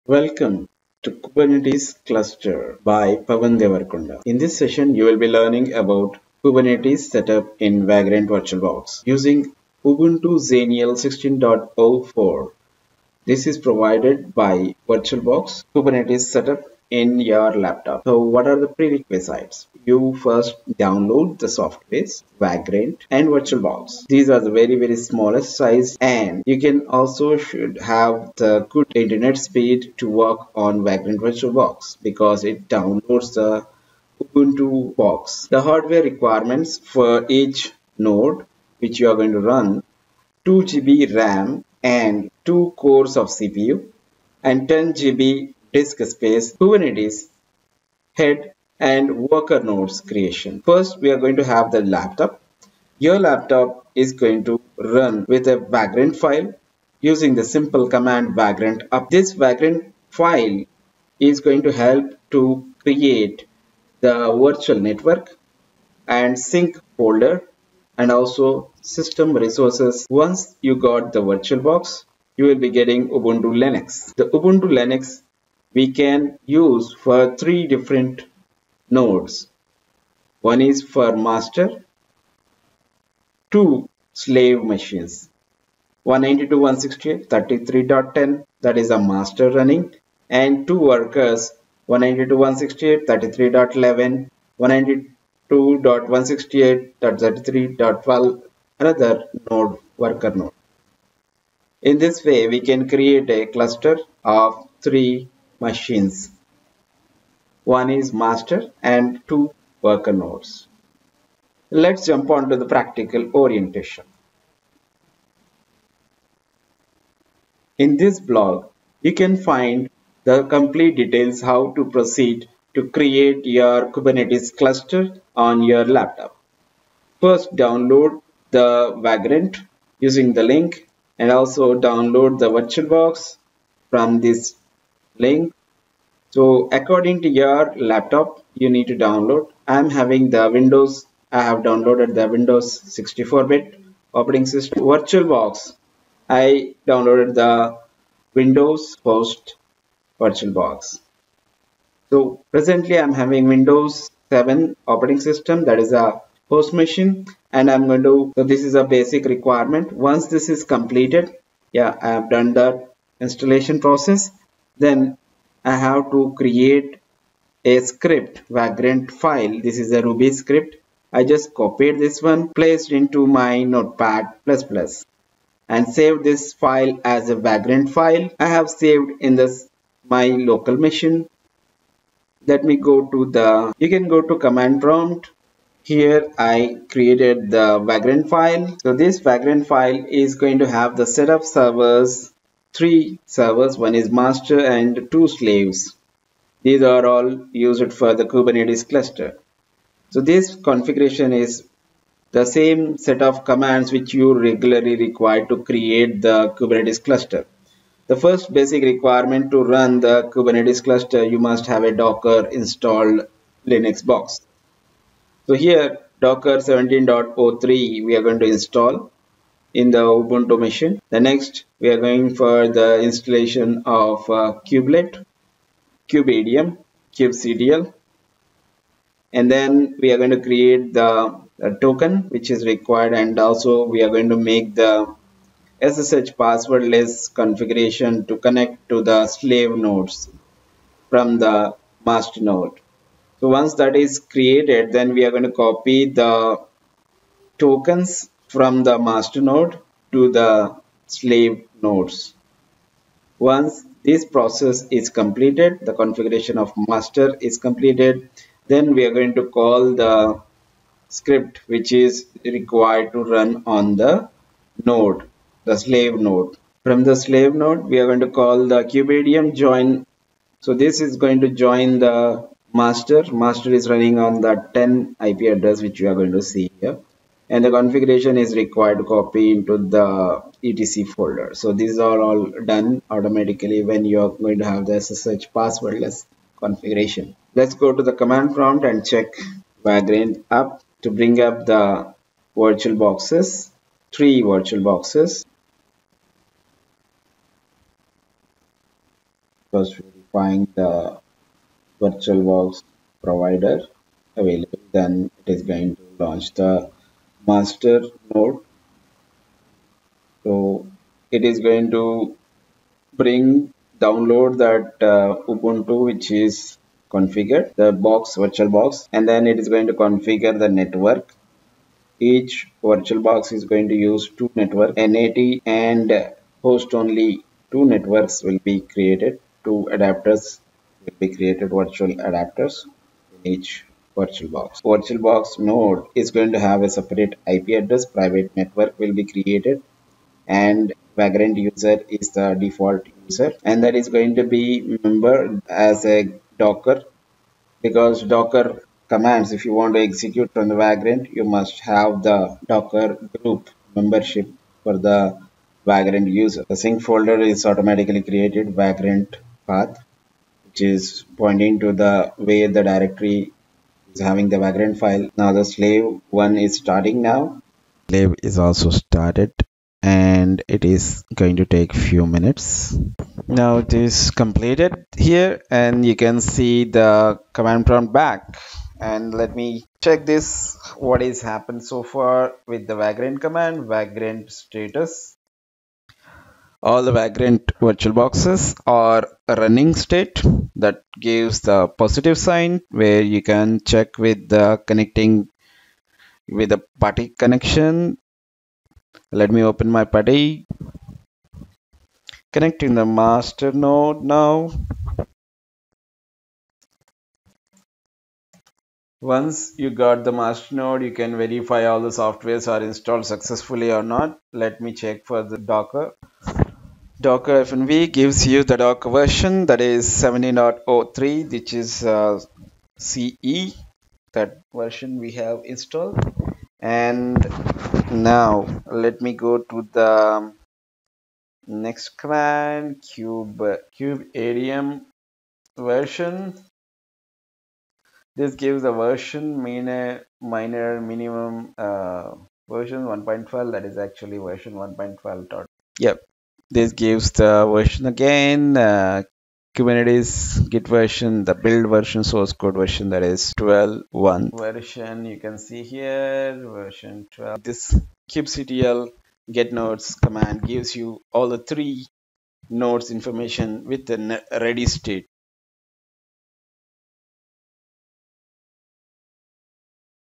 Kubernetes setup in your laptop with Vagrant VirtualBox
The commands used in this session
Please visit our blogs:

vagrant up
vagrant status

Software installed in Ubuntu boxes
docker -v
kubeadm -v
kubectl -v
kubelet --version

Confirmation of this k8s setup on your Master node
kubectl get nodes

All should have ready state